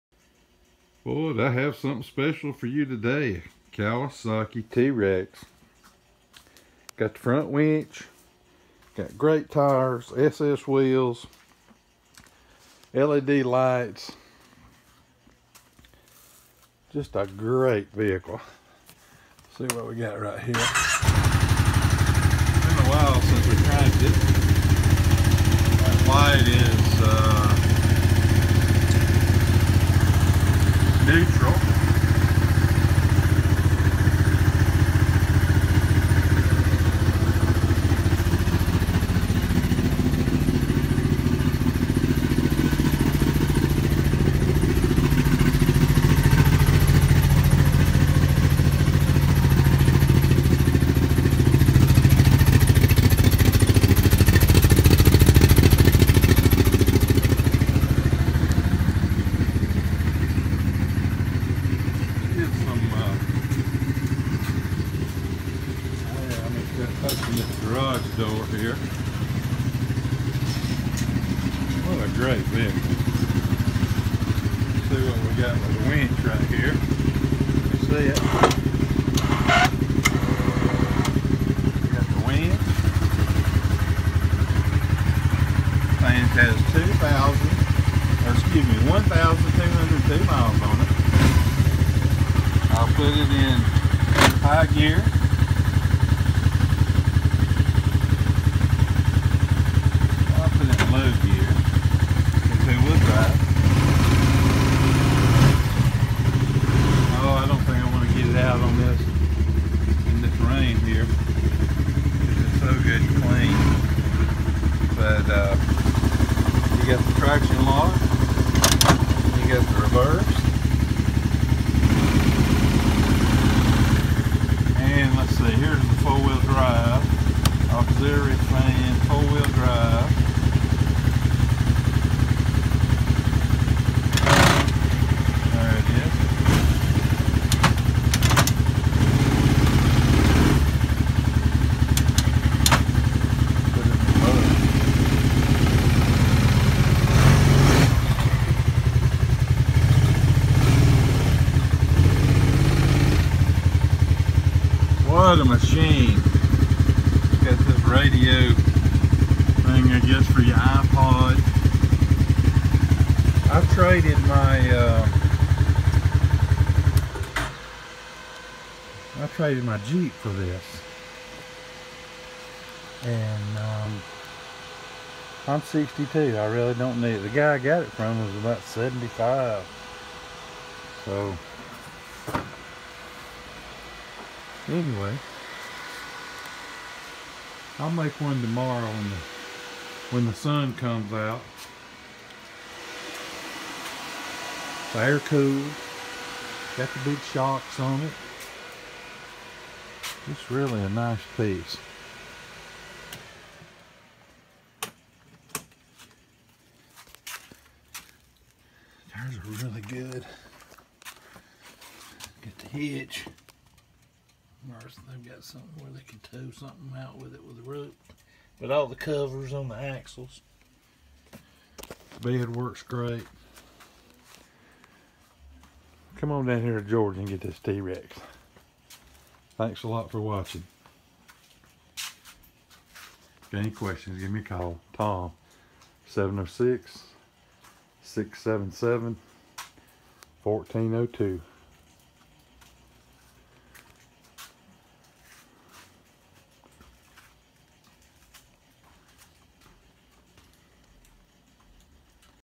Nice Teryx 750 Kawasaki 4x4 1202 Miles Green Black Winch SS Wheels Tomjonesinc1
Nice Teryx 750 Kawasaki 4x4 1202 Miles Green Black Winch SS Wheels
SAVE BIG !!! Just in time for Christmas.
On Craigs List Now $5950

SEE Youtube Video of Road Test and walk around. ( save link below to your browser )

Many Photos so please allow time (1 Minute) to load.
This 2008 UTV starts easily, Runs, Drives Smooth & Stops Great!!! Only 1202 Miles, Lots of Extras.
Yellow light on dash was Emergency brake was on during video, it works great, also is Water Cooled.
I have driven this UTV over my farm in the last few days and enjoyed every minute !!!
Equipment includes:
Kawasaki Gasoline Engine, Automatic Transmission, Rear 2 & 4 Wheel Drive with Low and High Range and Diff Lock, Speedometer,Temp-Oil-Volt Light, 4 X Matching tires Big on rear with 95%+ Tread-wear Remaining, Anti-Freeze to - 40 Degree
Sold As IS Where IS with NO Warranty Expressed or Emplied.
If this ad is on CL then UTV is Still For Sale.
Call or Tex Tom 7 O six six 5 8 five O 5 Too
Negatives are: NICE but Not Perfect. Small dents, dings, scrapes.
You Will Love This Truck !!!
This is the Most Popular Cargo Size UTV Truck and will be a Great Asset to any Farm or Campsite!
tomjonesinc1 TJINC